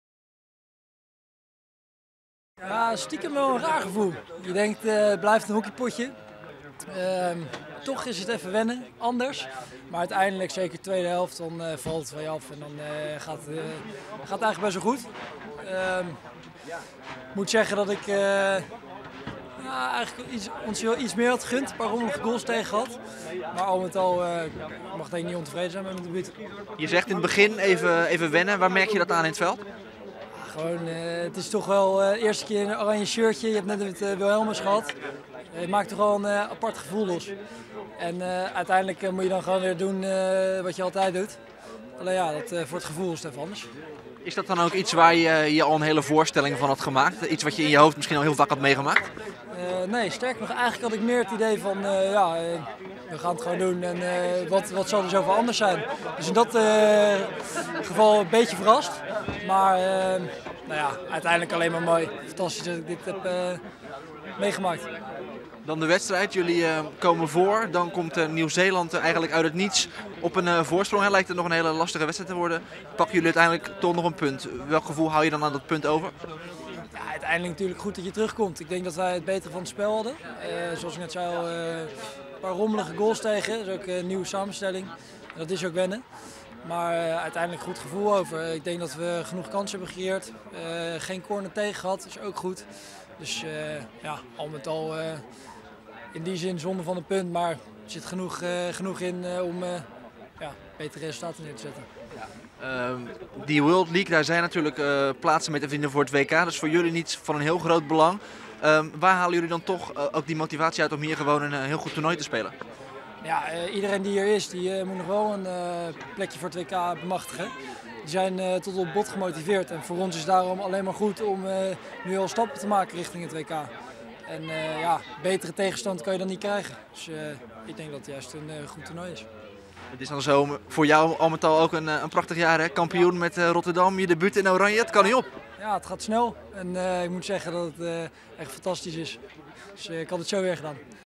Ook HCR-speler Hidde Turkstra debuteerde
De Nederlandse hockeyers zijn met een 3-3 gelijkspel tegen Nieuw-Zeeland begonnen aan de halve finale van de Hockey World League in Rotterdam. HCR-speler Jeroen Hertzberger maakte twee treffers. Zijn ploeggenoten Olivier Polkamp (foto) en Hidde Turkstra maakten hun officiële debuut in het Nederlands elftal.

   Lees meer: Oranje-debuut HCR-spelers met remise